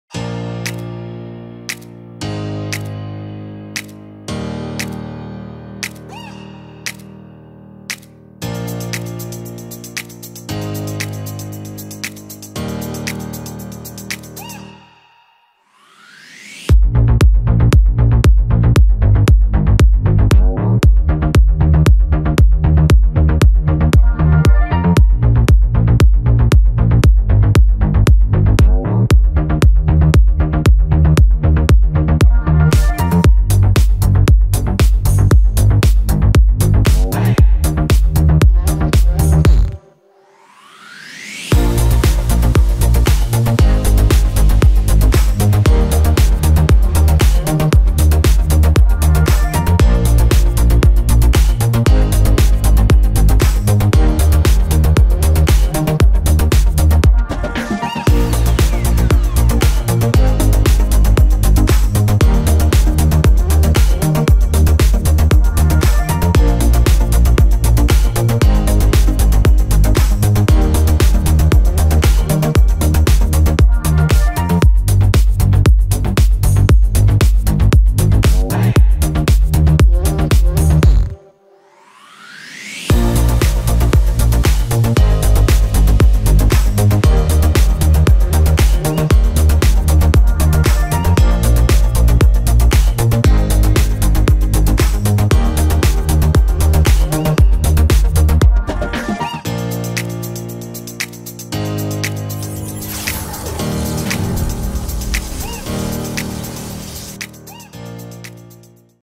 Félpályán Nyíregyházán
Átmenetileg a Tiszavasvári, a Korányi Frigyes és a Kótaji úton haladhat félpályán a forgalom. Bővebben itt: